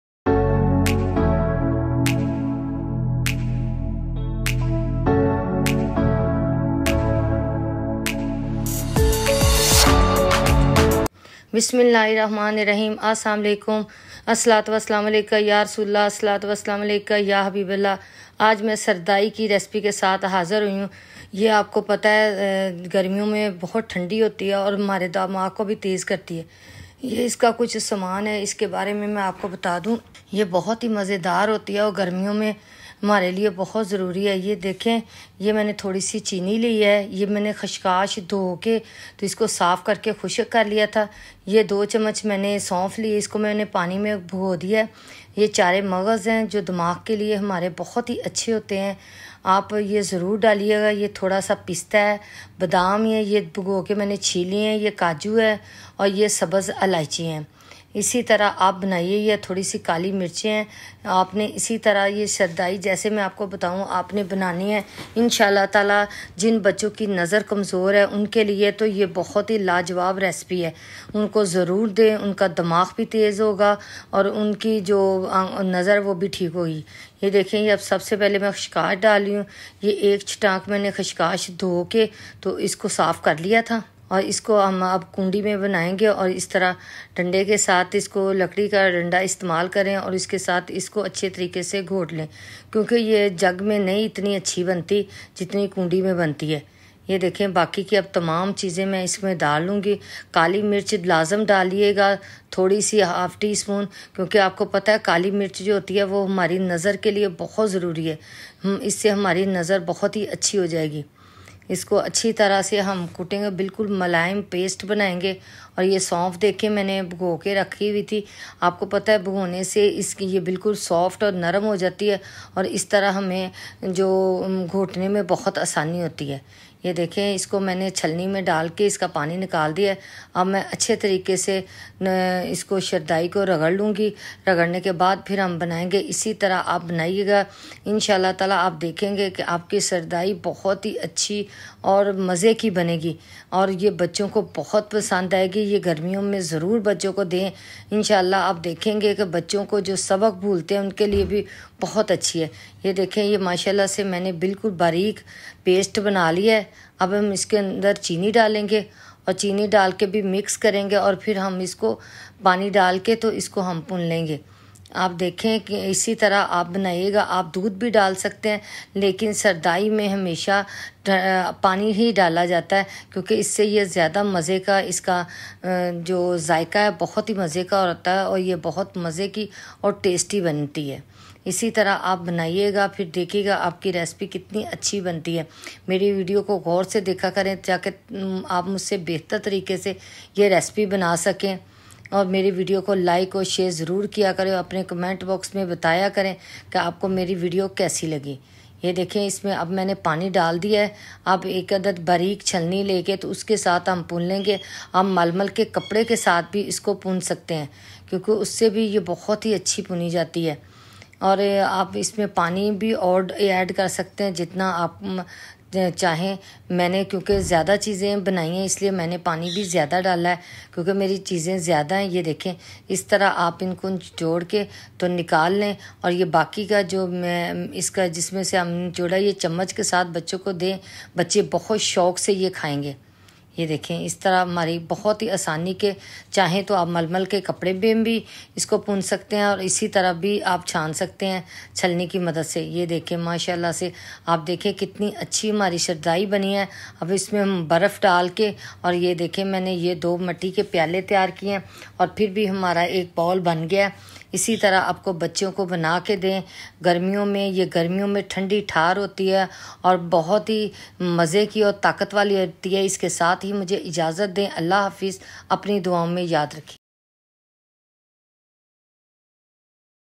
Sardai Recipe | Traditional Drink | Thandai Recipe | Dasi Amazing Drink
"Shardai Recipe: A fusion of bold flavors and cultural heritage, combining tender lamb simmered in aromatic spices with creamy coconut milk and a dash of tangy tamarind. Served over fragrant jasmine rice, it's a tantalizing journey for the taste buds."
چینی=حسب ضرورت
خشخاش=3چمچ
سونف=2چمچ
ڈرائی فروٹ=حسب ضرورت

Keep Supporting me So I can Continue to provide you with free recipes each week!!